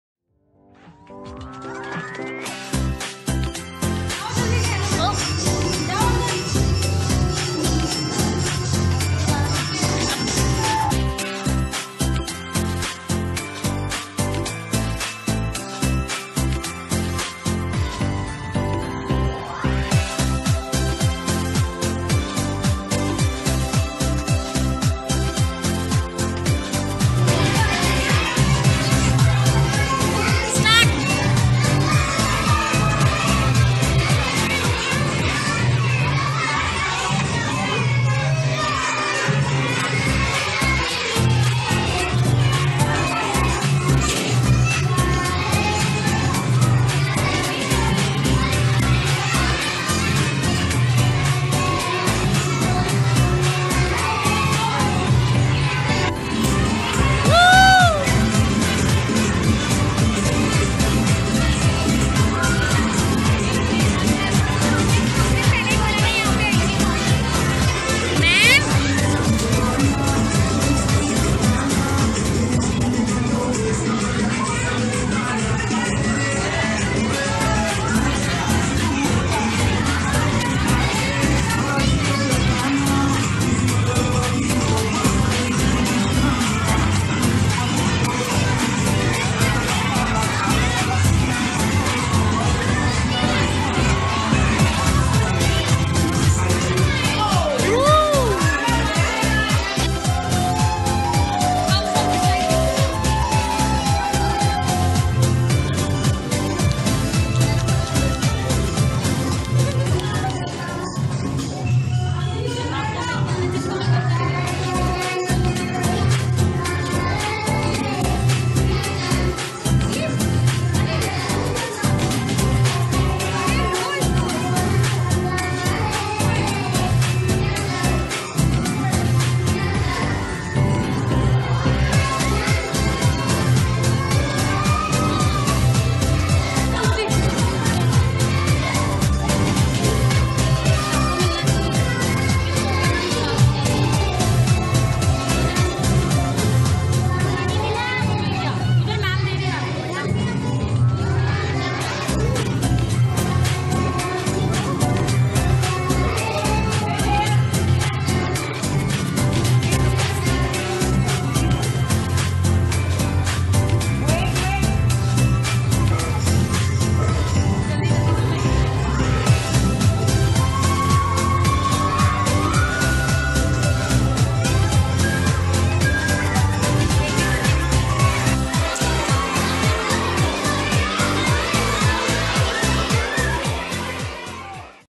Children’s Day @ Vagishwari world school 2019
As the saying goes, ‘there is a child in each one-off us’.
Children’s Day holds a special place for everyone. The students and teachers of Vagishwari world school celebrated the occasion and the birthday of Pandit Jawaharlal Nehru with utmost joy and excitement.The importance and significance of this special day was also explained to the young children. There were a lot of surprises for the children too. There were some special performances by the teachers which was highly applauded by the young ones and the students were overwhelmed. It was a day happily spent and children went back with happy memories.